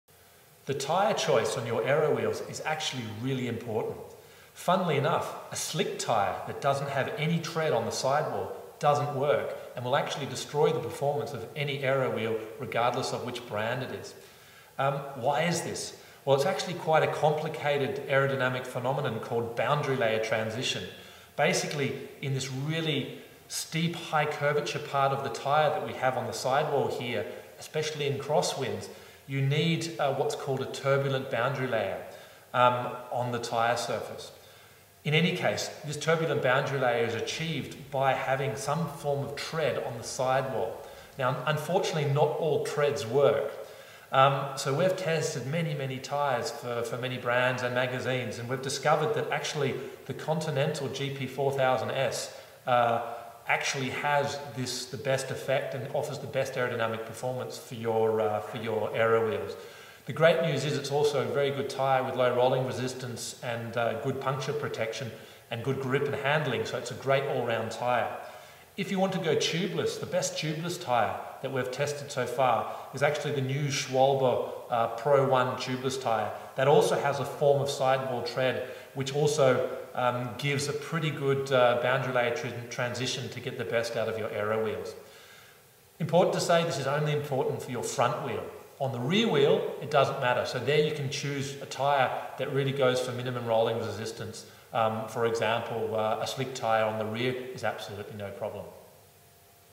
Swiss Side Aero Tip - Tyres
Swiss Side's technical director, Jean-Paul Ballard talks about the effect of your tyres on your aerodynamic performance. Every WATT counts.